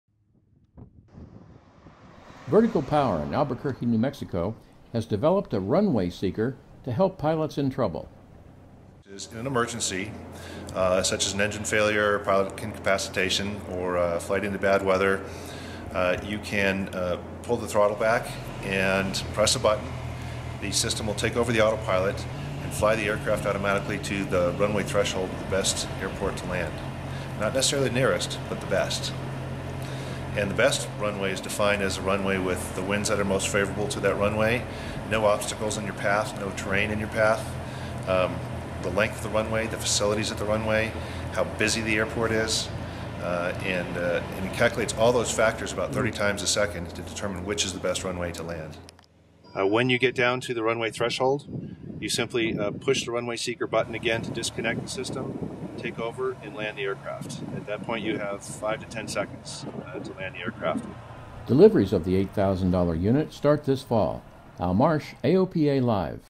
Automate your deadstick landing
Vertical Power in Albuquerque has developed a system to take charge of your aircraft in an emergency and fly it to the approach end of a runway. Just push the red button! Here's the ling